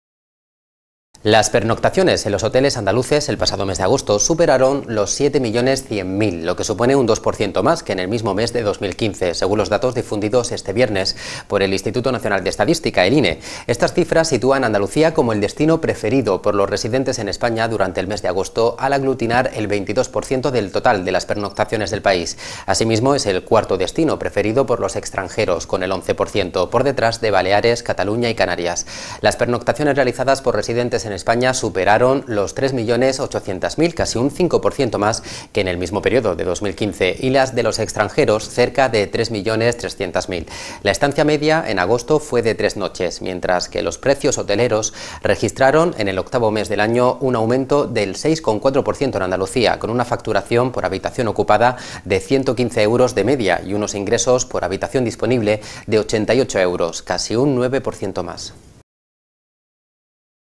Málaga 24h TV - Las pernoctaciones hoteleras en Andalucía aumentan un 2% en agosto
Las pernoctaciones en los hoteles andaluces el pasado mes de agosto superaron los 7 millones 100 mil, lo que supone un 2,0 por ciento más que en el mismo mes de 2015, según los datos difundidos este viernes por el Instituto Nacional de Estadística (INE).
Estas cifras sitúan a Andalucía como el destino preferido por los residentes en España durante el mes de agosto al aglutinar el 22 por ciento del total de las pernoctaciones del país. Asimismo, es el cuarto destino preferido por los extranjeros, con el 11 por ciento, por detrás de Baleares, Cataluña y Canarias.
Las pernoctaciones realizadas por residentes en España superaron los 3 millones 800.000, casi un 5 por ciento más que en el mismo periodo de 2015; y las de los extranjeros, cerca de 3 millones trescientas mil.

English:

Overnight stays in hotels in Andalusia last August exceeded 7 million 100 thousand, which is 2.0 percent more than in the same month of 2015, according to figures released Friday by the National Statistics Institute (INE ).
These figures place Andalusia as the preferred destination for residents in Spain during the month of August to bring together 22 percent of the total overnight stays in the country. It is also the fourth preferred by foreigners, with 11 percent, behind the Balearic Islands, Catalonia and the Canary Islands destination.
Overnight stays by residents in Spain topped three million 800,000, nearly 5 percent more than in the same period of 2015; and foreigners, about 3 million three hundred thousand.